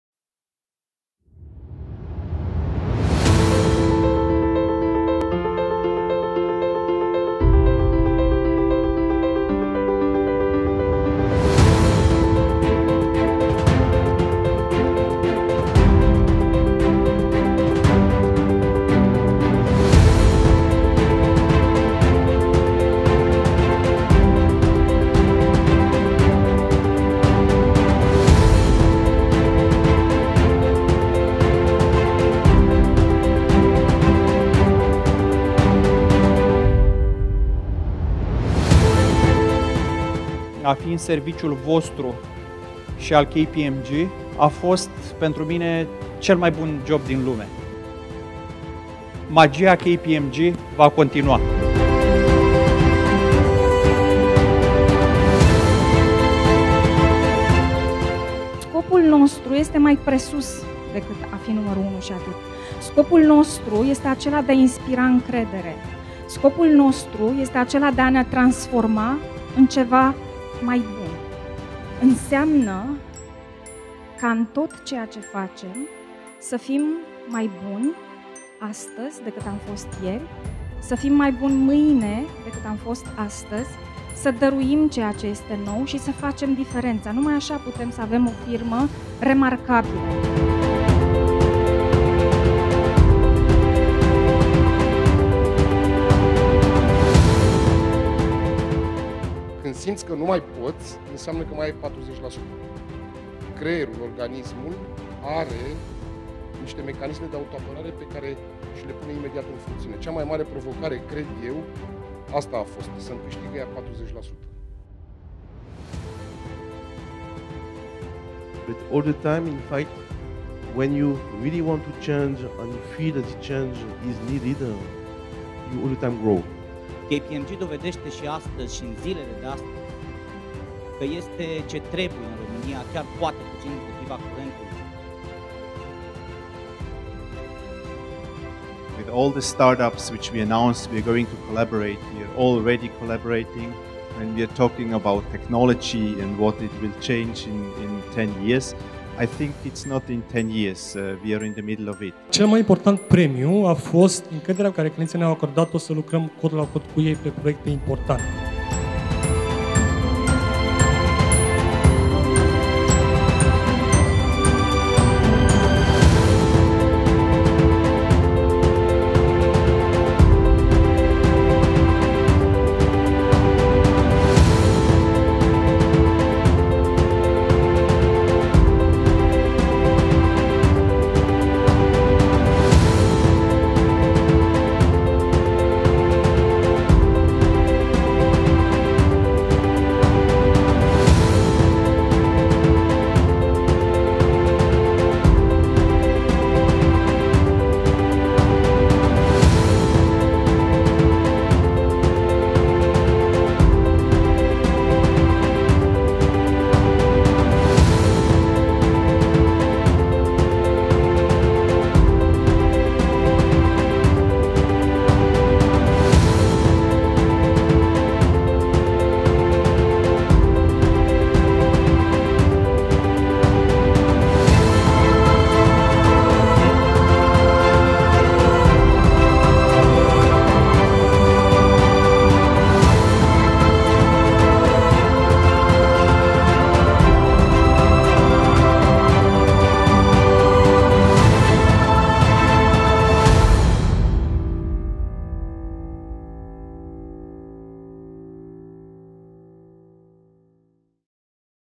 2018 KPMG Gala
About trust, talented people, and sustainable growth at the 2018 KPMG Gala.